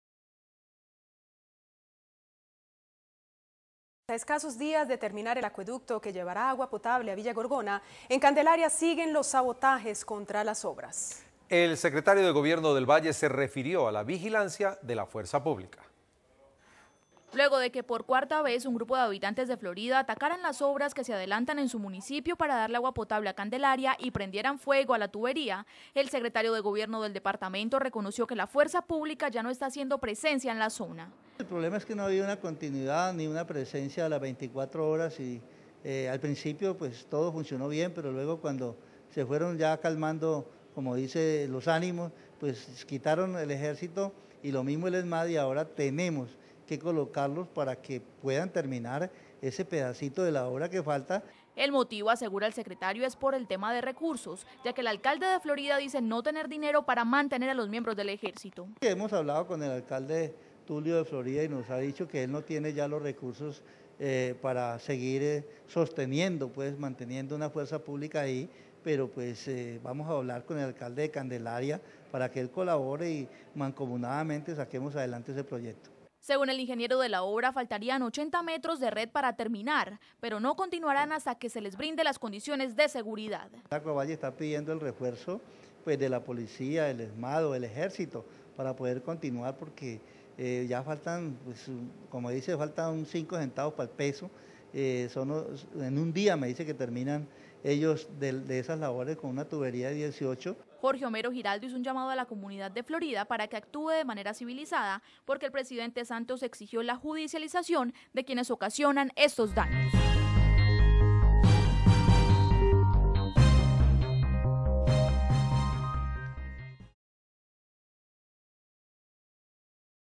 Junio 3 de 2014. Continúan los sabotajes contra las obras de acueducto de Villagorgona
A escasos días de terminar el acueducto que llevará agua potable a Villagorgona, en Candelaria siguen los sabotajes contra las obras. El Secretario de Gobierno del Valle se refirió a la vigilancia de la Fuerza Pública.